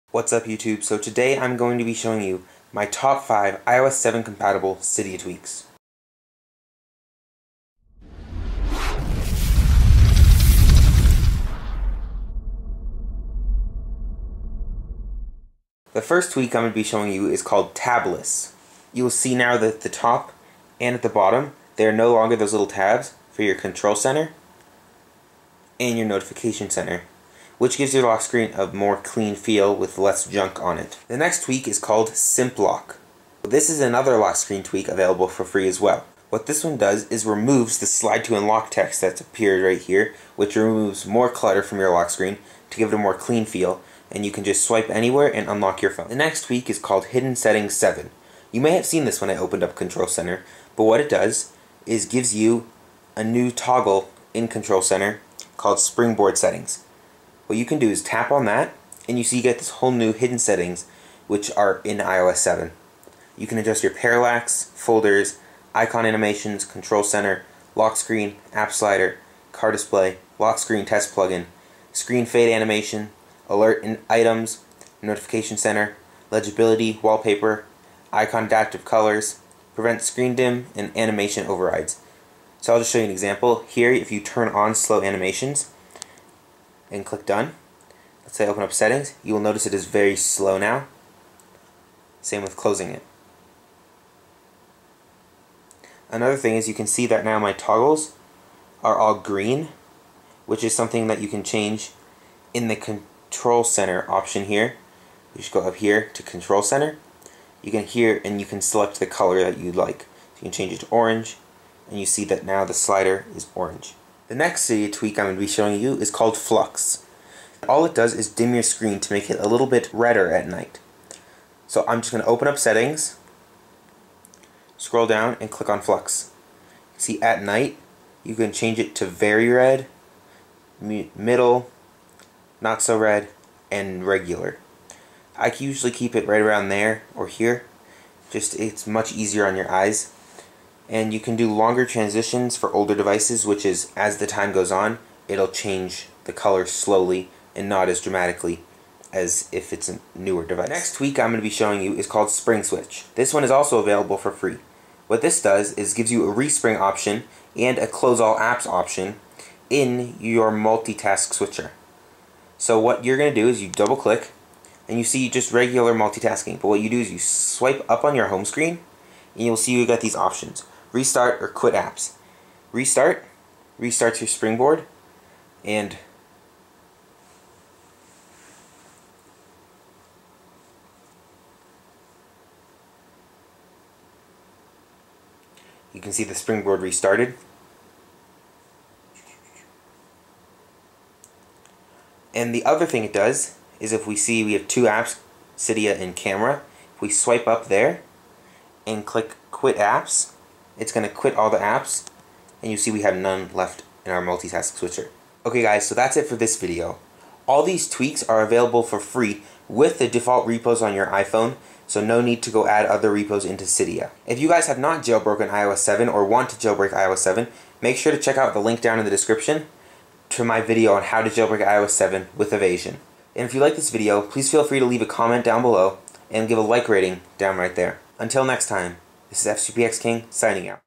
iOS 7 Compatible Cydia Tweaks
This blog has up-to-date info on the latest tech news and rumors!

Tweaks:
1) Tabless
2) Simplock
3) Springboard Settings
4) F.lux
5) Spring Switch
Note: All these tweaks are free and from the default repos on your iPhone